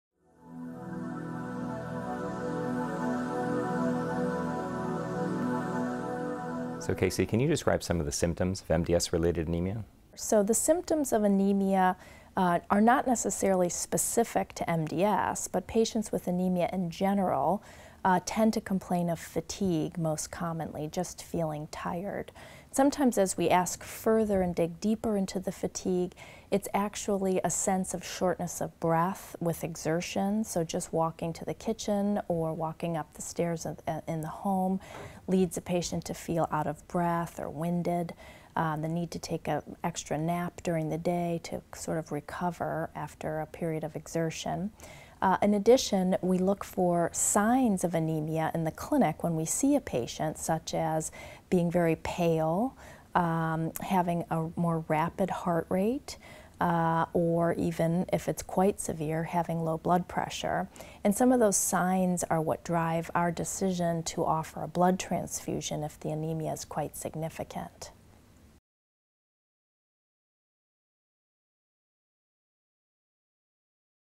What are the symptoms of MDS-related anemia?
MDS experts, Dr Rafael Bejar and Dr Casey O’Connell, provide an overview of the symptoms of MDS-related anemia. The symptoms of anemia are not necessarily specific to MDS. In general, the most common symptoms include fatigue, shortness of breath, and the need for rest following physical exertion. Other symptoms include being pale, having a more rapid heart rate, and in severe cases having low blood pressure.